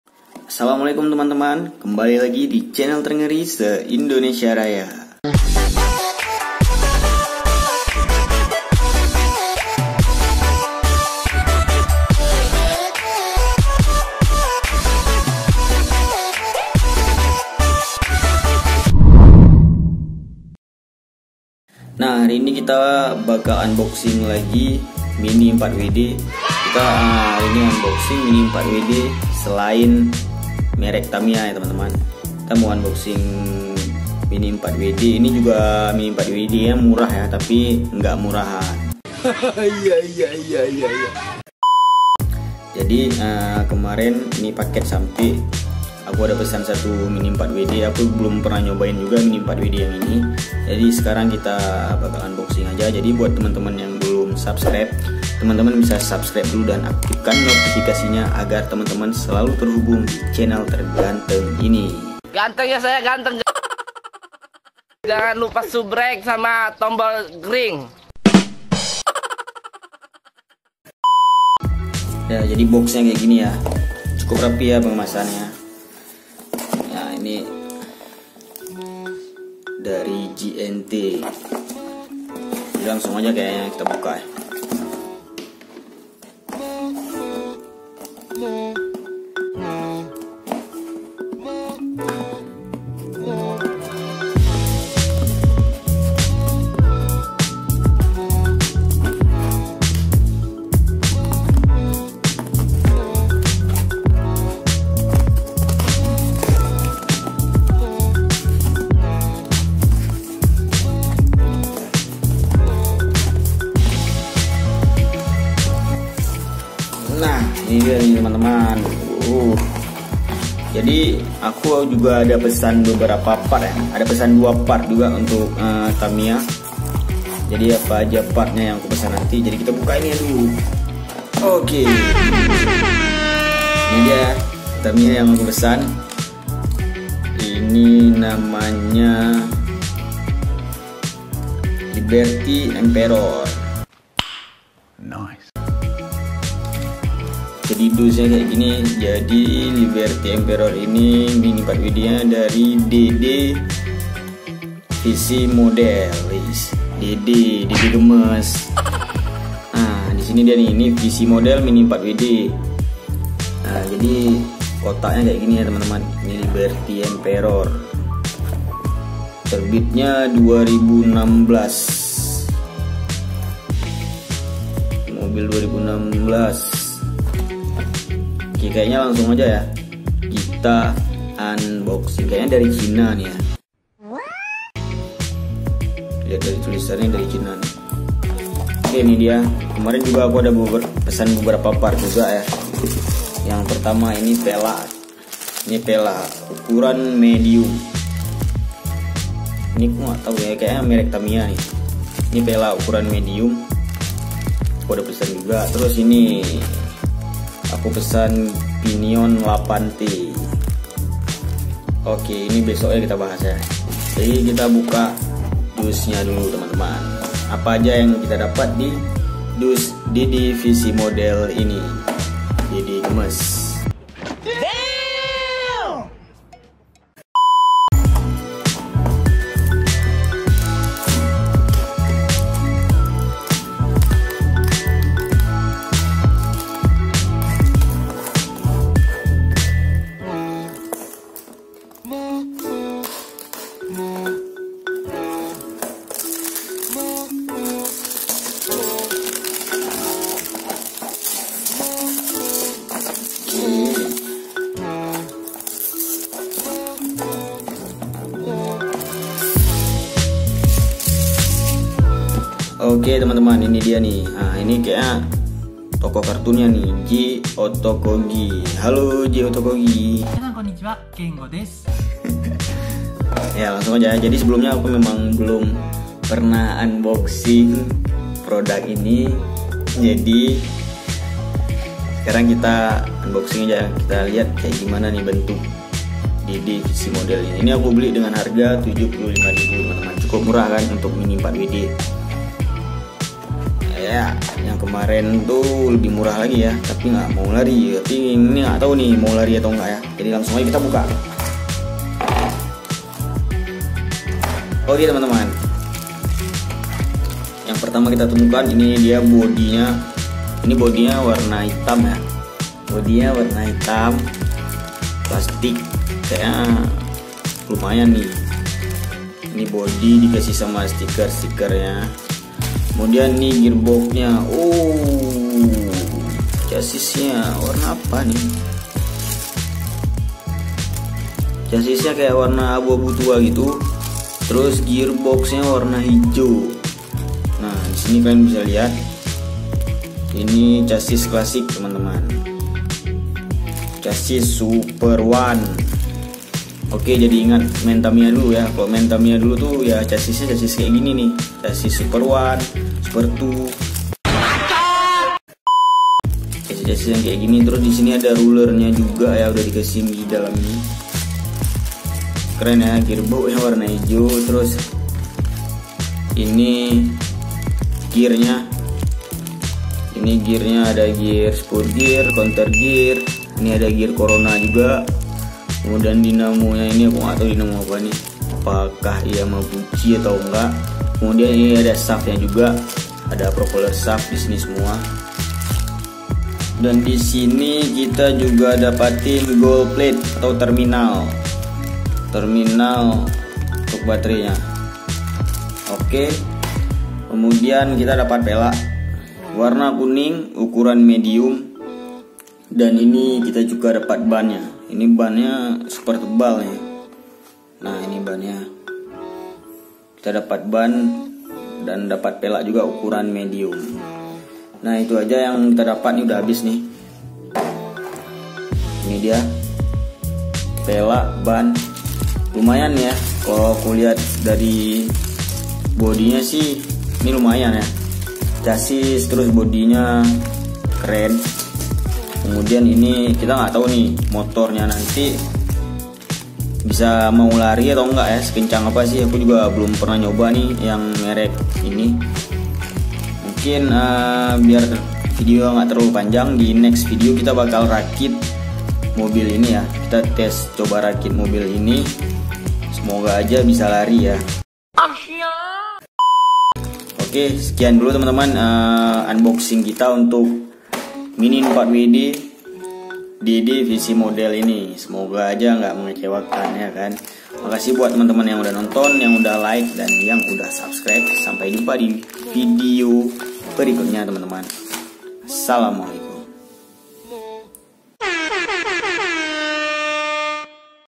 Unboxing Mini 4WD klasik, Nostalgia bareng Chassis tersebut
Kembali mencoba Mini 4WD yang baru pertama kali kita Unboxing, semoga aja hasilnya bagus, setidaknya sesuai harga, intinya tetap terus belajar.
Lokasi Banda Galaxy
Alamat : Banda Aceh, Indonesia